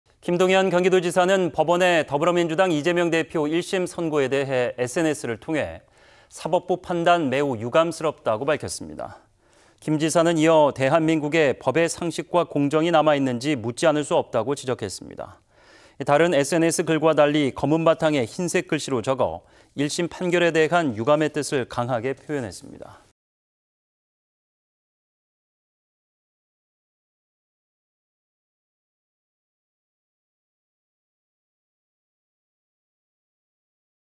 김동연 "사법부 판단 매우 유감"... 이재명 1심 판결에 SNS 글 올려
김동연 경기도지사는
법원의 더불어민주당
이재명 대표
1심 선고에 대해 SNS를 통해
"사법부 판단,
매우 유감스럽다"고
밝혔습니다.

김 지사는 이어 "대한민국에 법의 상식과
공정이 남아 있는지 묻지 않을 수 없다"고
지적했습니다.

다른 SNS 글과 달리
검은 바탕에 흰색 글씨로 적어 1심 판결에 대한 유감의 뜻을 강하게 표현했습니다.